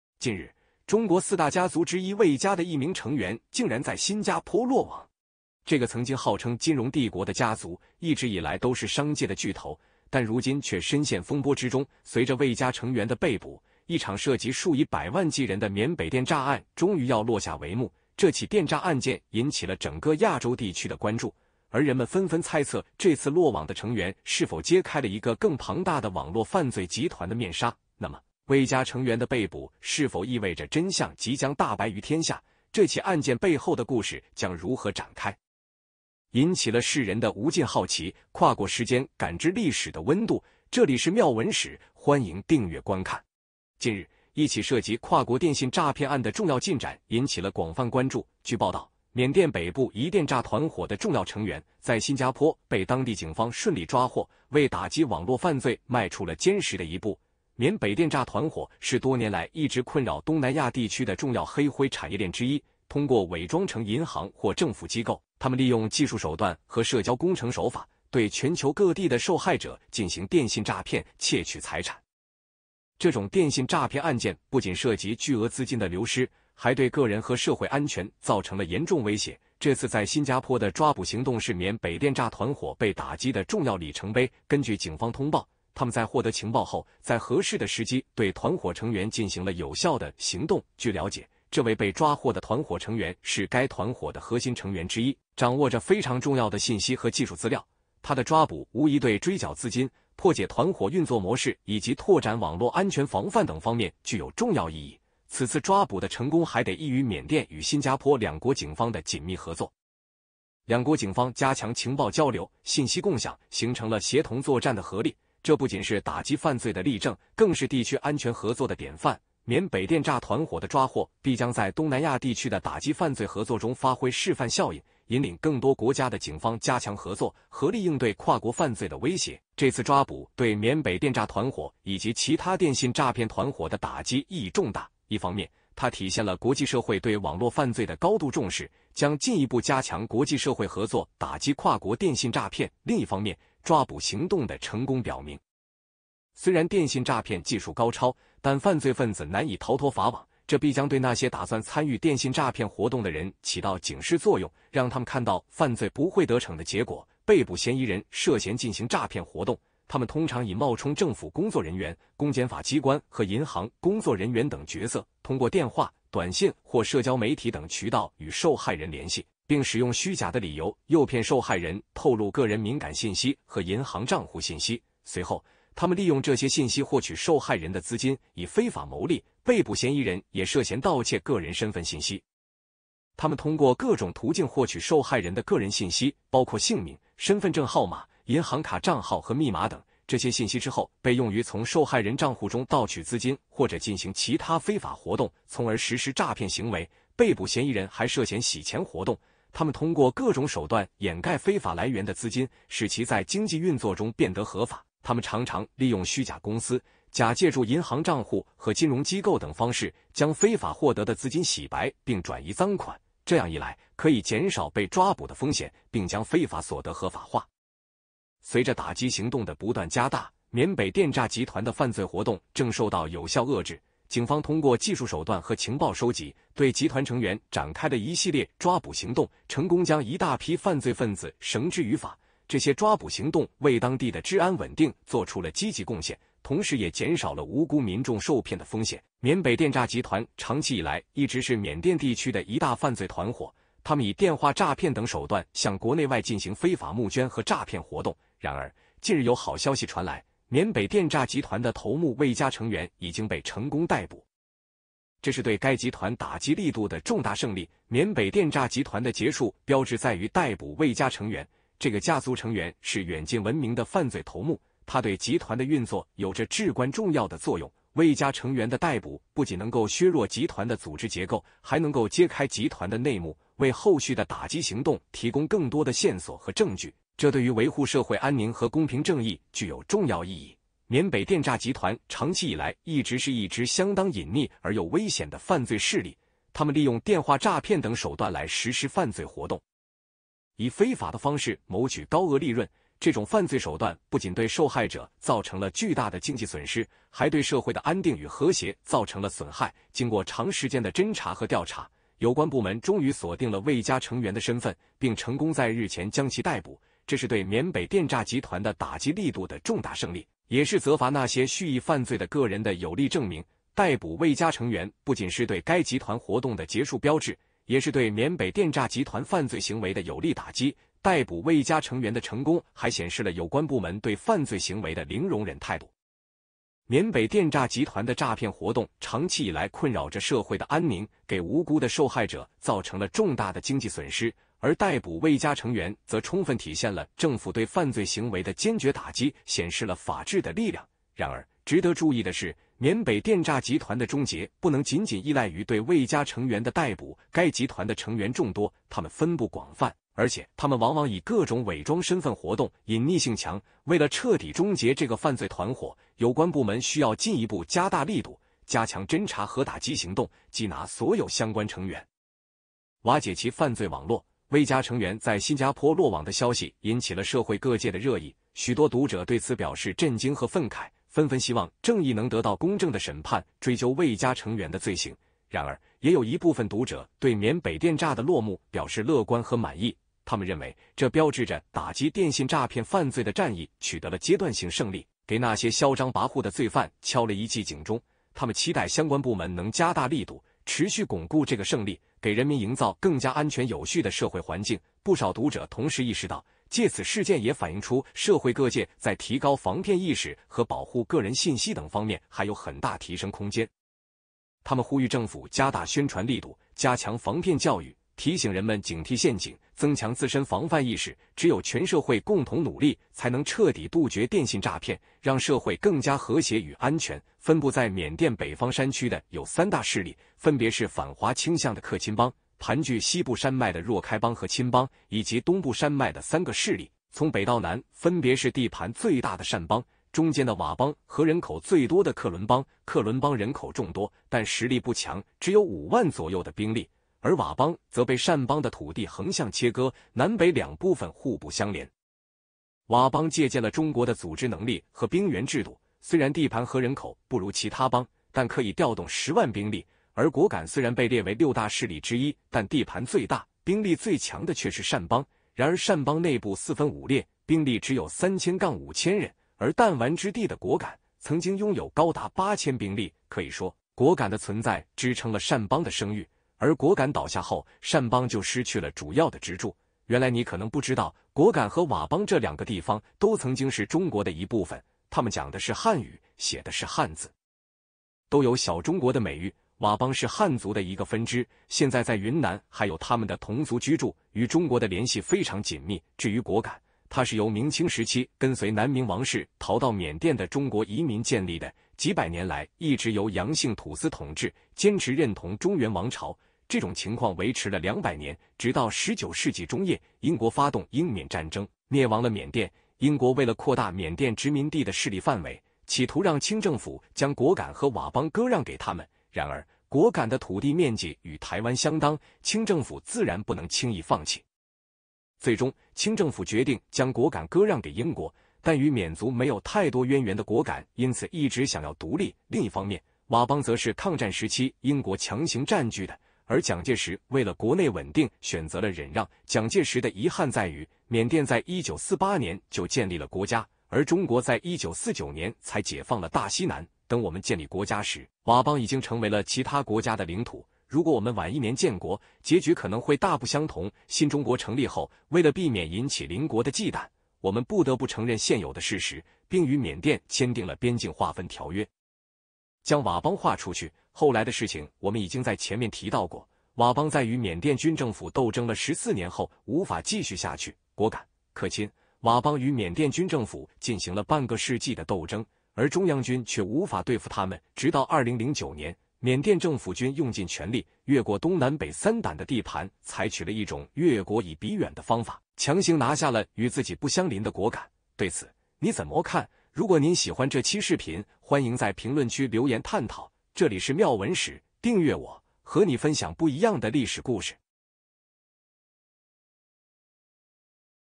四大家族之一魏家有人在新加坡落網，緬北電詐正在落下帷幕？【妙文史】歷史
近日，中國四大家族之一魏家的一名成員竟然在新加坡落網！這個曾經號稱「金融帝國」的家族，一直以來都是商界的巨頭，但如今卻深陷風波之中。隨著魏家成員的被捕，一場涉及數以百萬計人的緬北電詐案終於要落下帷幕。這起電詐案件引起了整個亞洲地區的關註，而人們紛紛猜測這次落網的成員是否揭開了一個更龐大的網絡犯罪集團的面紗。那麼，魏家成員的被捕是否意味著真相即將大白於天下？這起案件背後的故事將如何展開，引起了世人的無盡好奇。